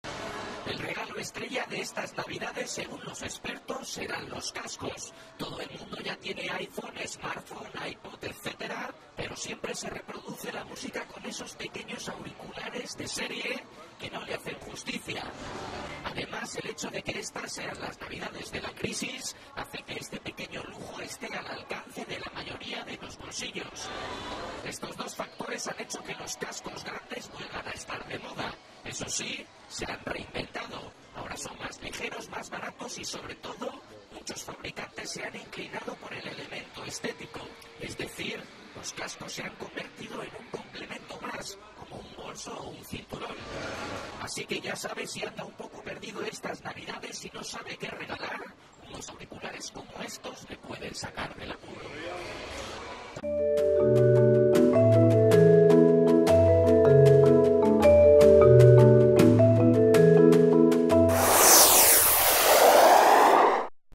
El regalo de estas navidades
Redacción, 22 de diciembre (CERESTV.info)

Están de moda y se han convertido en un complemento más. Son la nueva generación de auriculares.